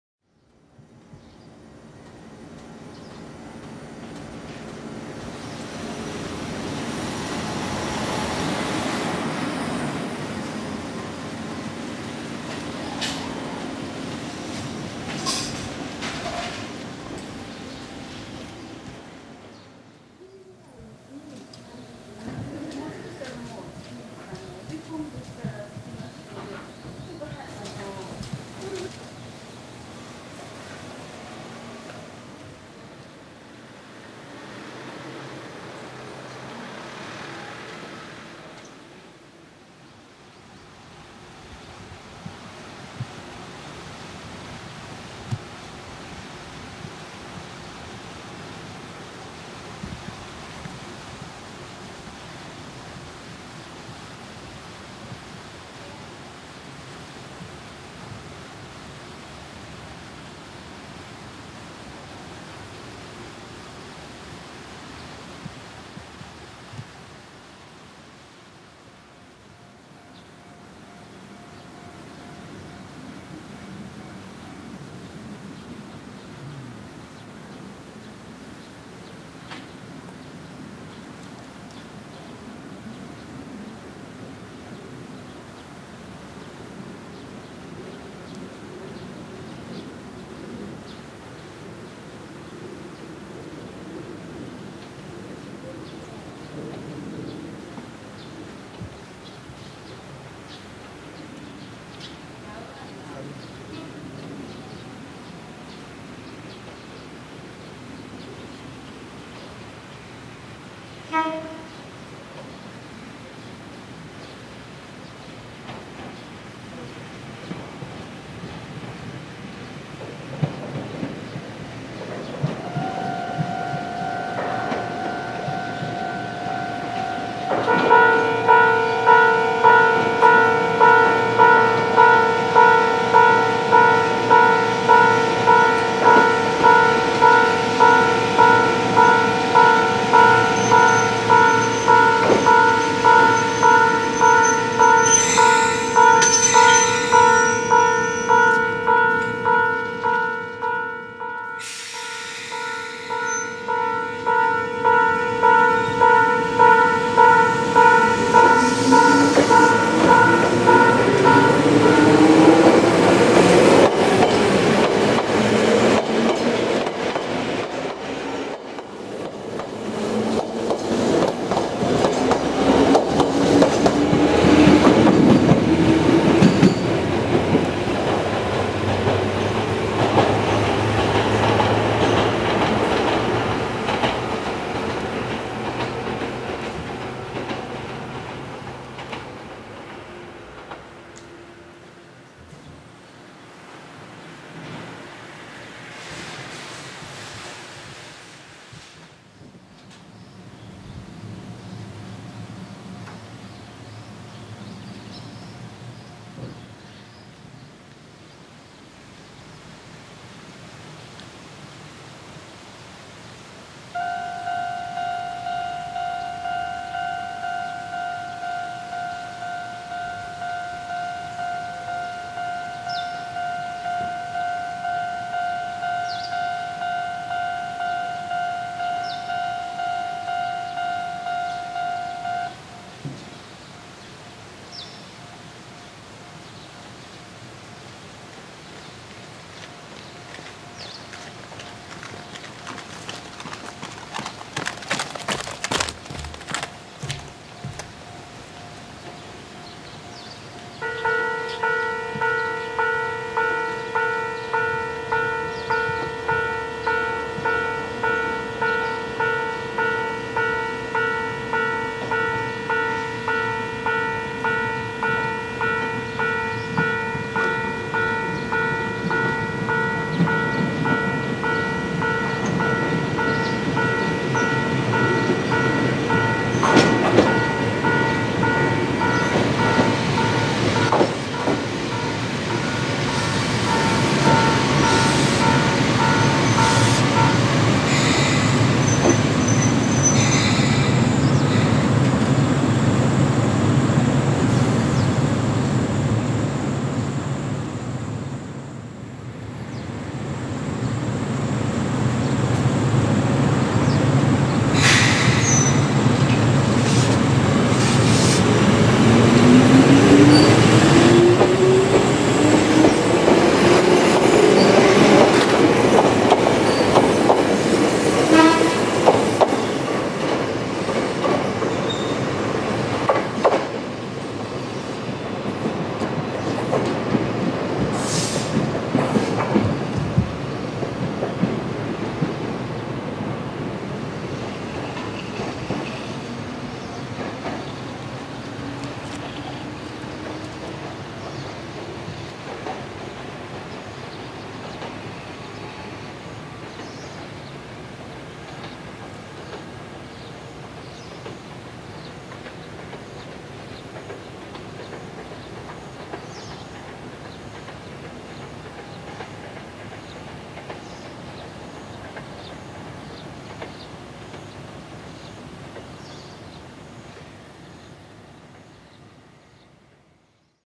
【とよさと！】近江鉄道豊郷駅 OumiRailway Toyosato station
けいおん！でおなじみ
豊郷小学校旧校舎群への最寄駅近江鉄道 豊郷駅です

Nearest to
"Toyosato elementary school old building"(animation K-ON!)
Oumi raiway toyosato station